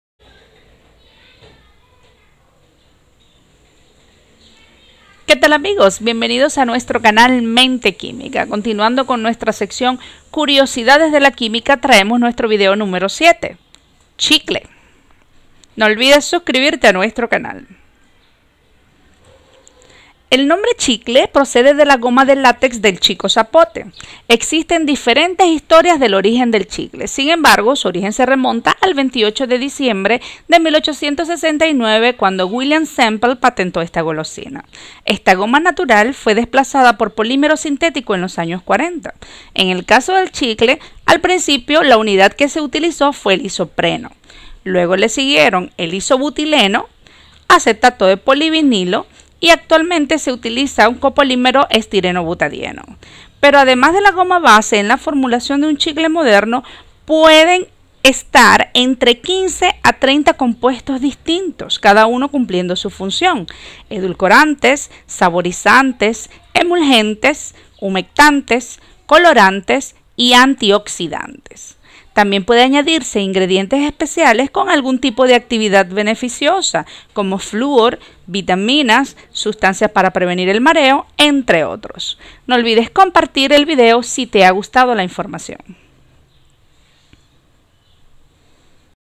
Curiosidades de la Química Nº7: Chicle.
Bienvenidos a nuestro canal MenteQuímik

Este es el vídeo numero 7 de nuestra sección llamada “Curiosidades de la Química”. Un  vídeo corto, claro y conciso que presentará tips de química con la fundamentación adecuada para que puedas impactar a tu público con tus conocimientos.